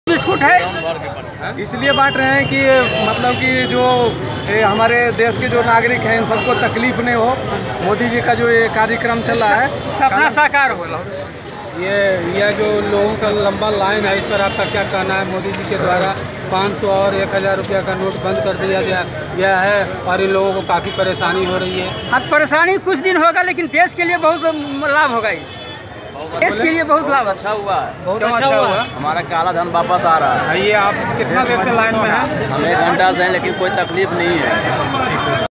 शेखपुरा(बिहार)-स्वदेशी स्वाभिमान मंच के कार्यकर्ताओं ने मोदी जी के कालाधन के खिलाफ अभियान में सहयोग
500 और 100 के नोट बन्द करने के बाद घंटों बैंक के कतार ने लगे लोंगों का हौसला कम नही, कालाधन वापस लाने के लिए सब सहने को तैयार हैं लोग